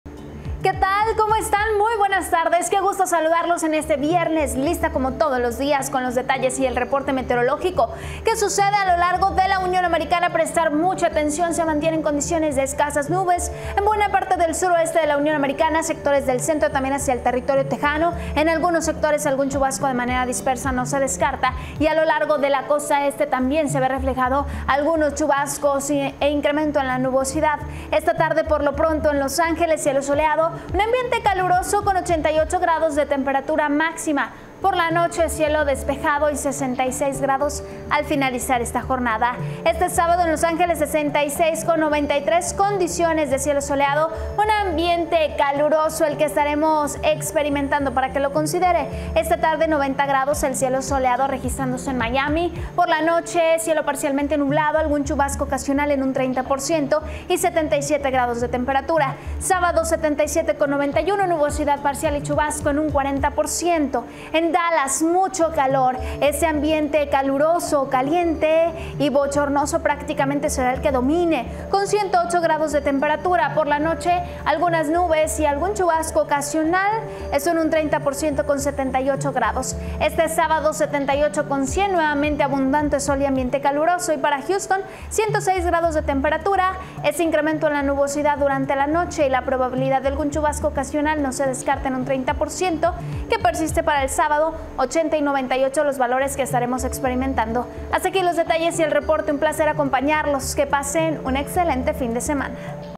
¿Qué clima nos depara de cara al fin de semana?
🇺🇸☀️ Se espera un clima seco en gran parte de los Estados Unidos; sin embargo, en algunas zonas del país se registrarán esporádicas lluvias.